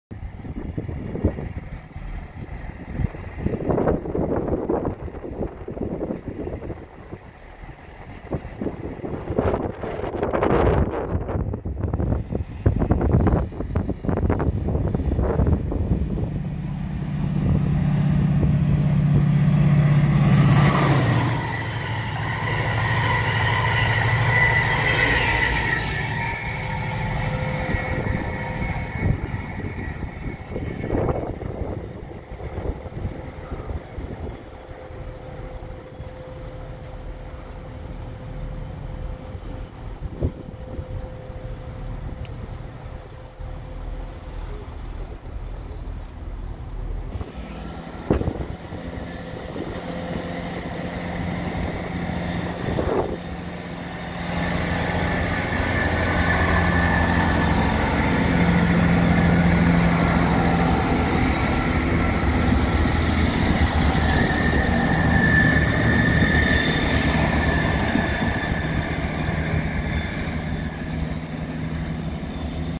Chiltern railway Dmu working the 2A28 service
class 165002  DMu working the 2A28 service from London Marylebone to Aylesbury chilten railway services